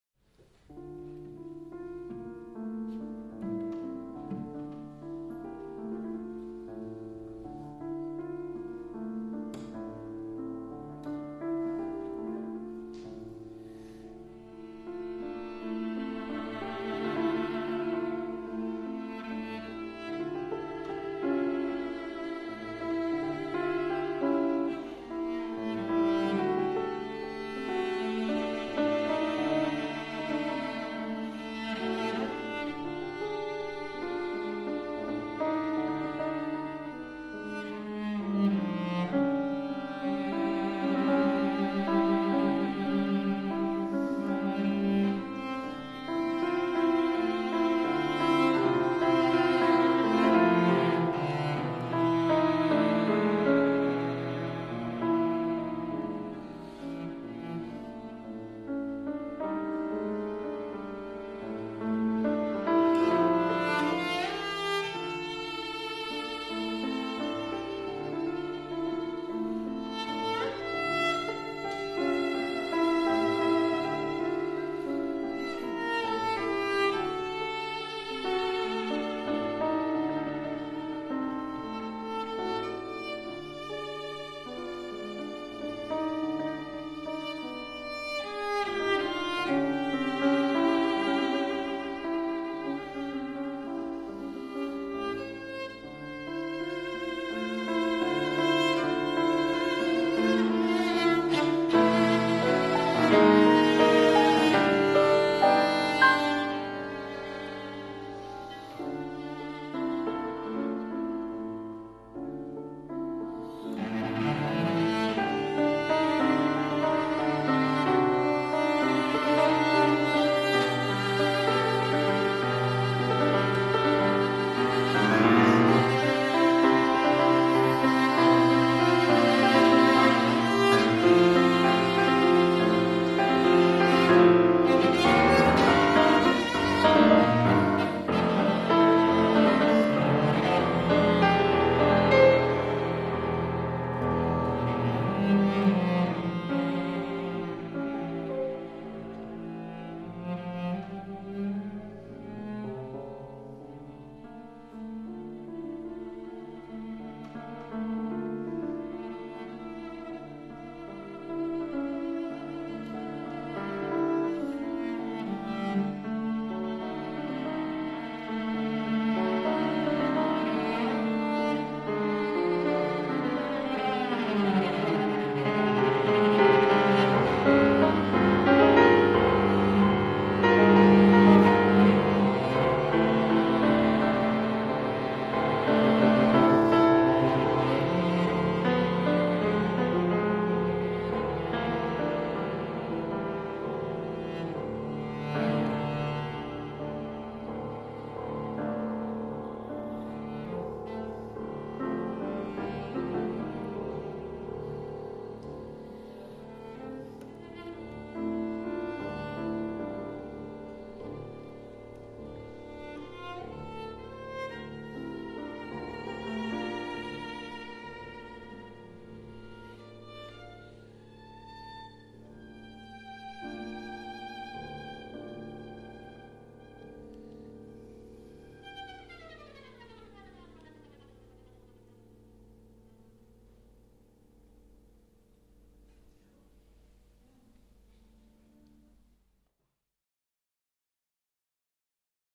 Natalie Clein and Natacha Kudritskaya in Rehearsal - Oxford Chamber Music Festival
Natalie Clein -cello and Natacha Kudritskaya -piano. Oxford Chamber Music Festival 2011.